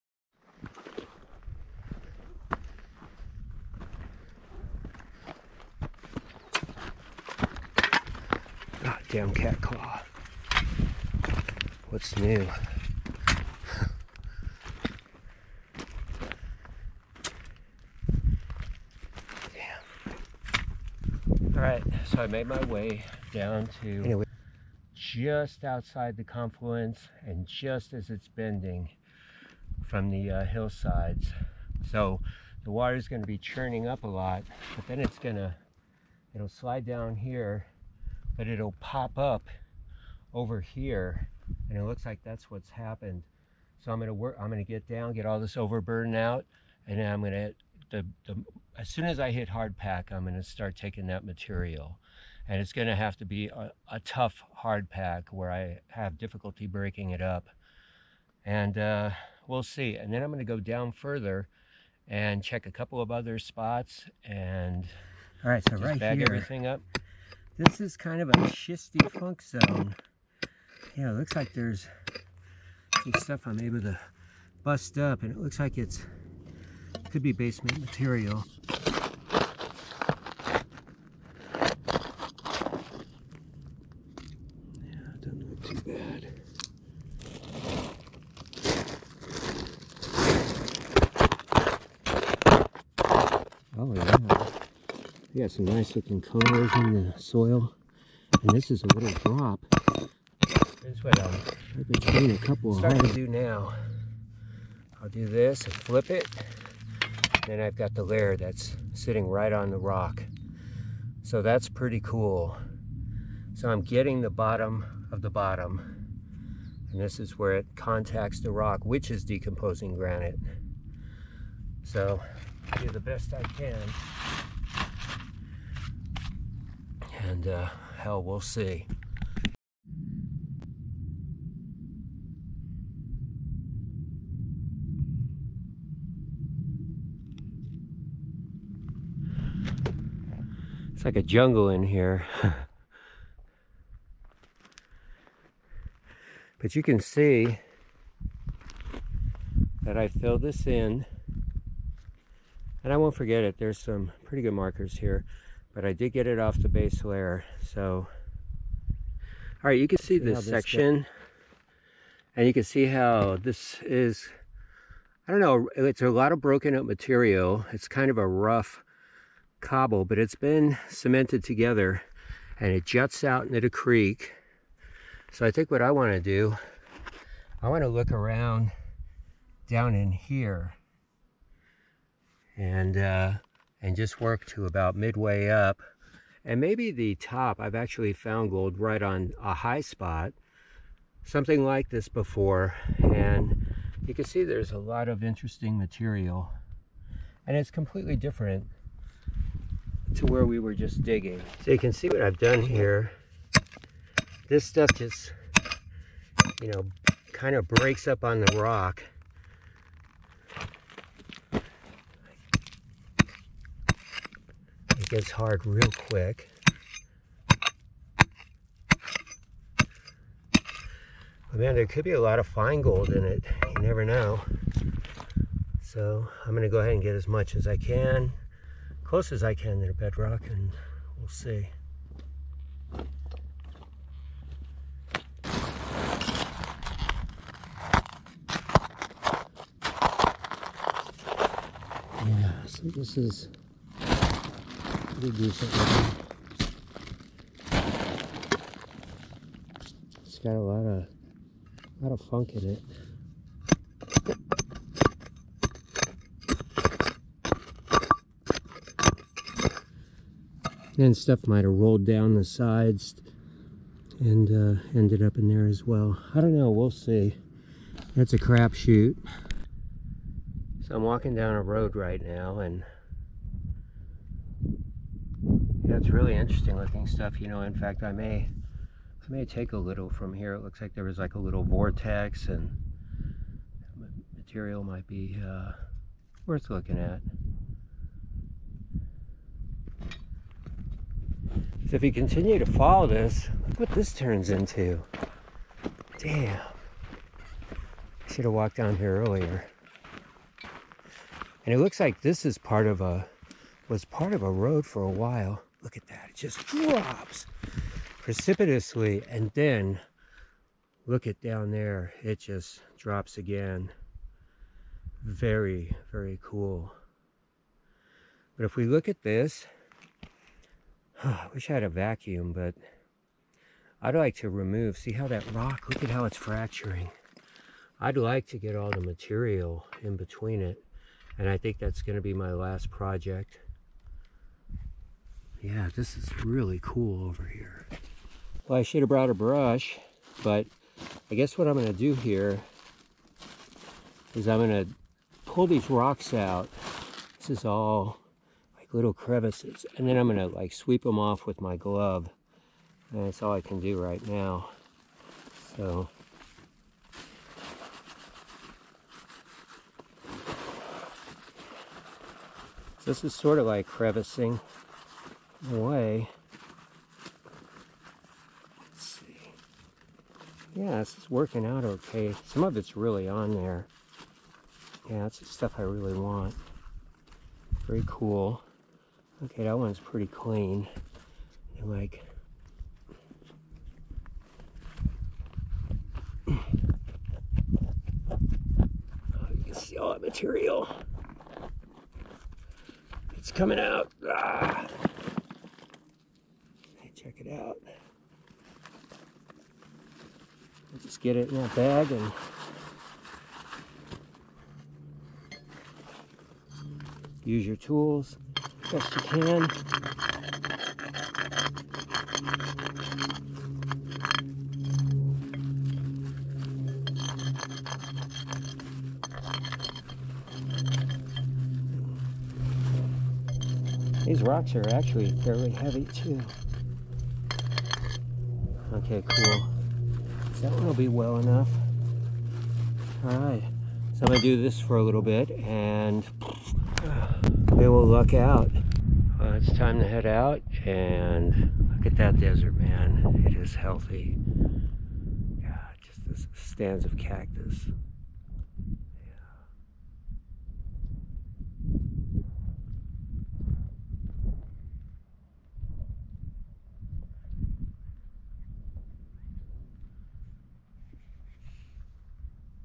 Chasing Gold near Cave Creek
A beautiful desert setting courtesy of a wet Winter. This is a follow-up search  for better gold values. Sampling and hiking the area was definitely good fun.